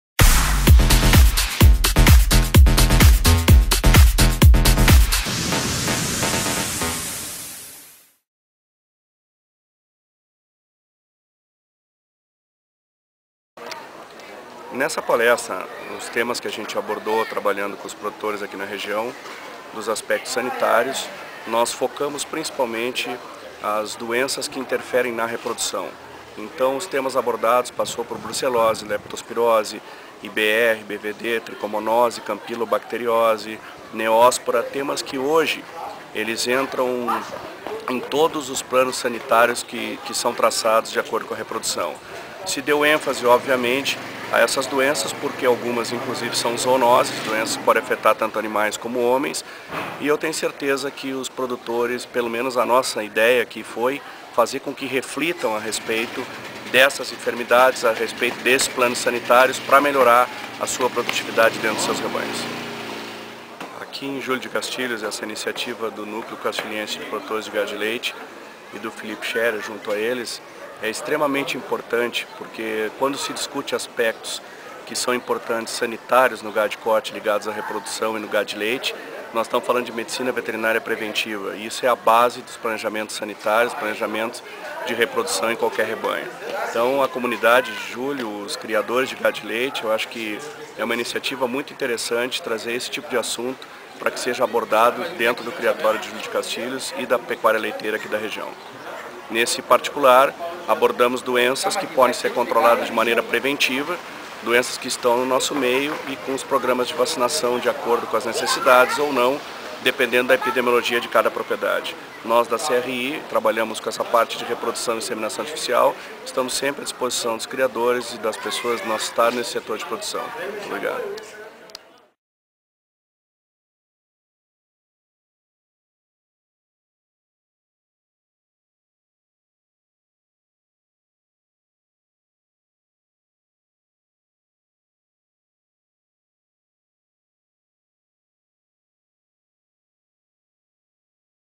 Dr. Marcelo M. Dias palestrou na 48ª EXPOJUC
Durante a realização da 48ª EXPOJUC em Júlio de Castilhos o correu a palestra sobre Aspectos Sanitários importantes no sucesso da reprodução em bovinos onde o palestrante foi o senhor Dr. manoel Maronna Dias o qual teve o reconhecimento pela sua participação neste grande evento.